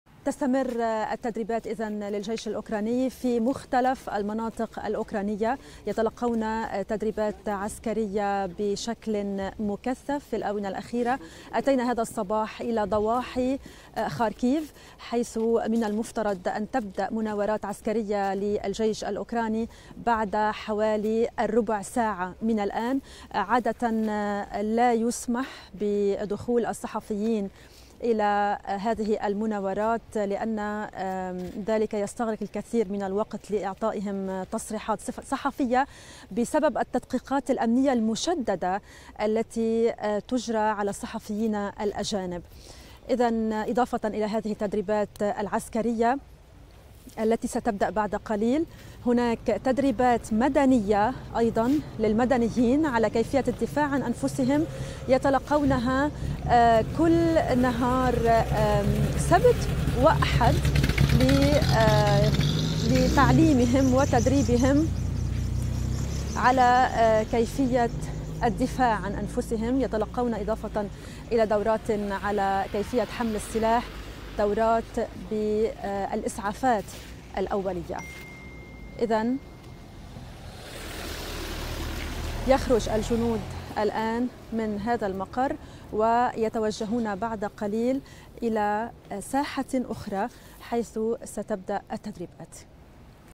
أوكرانيا تبدأ المناورات العسكرية تحسبًا للغزو الروسي المحتمل
تشمل المدنيين في عدة مناطق.. بدء مناورات عسكرية للجيش الأوكراني تحسبًا لأي غزو روسي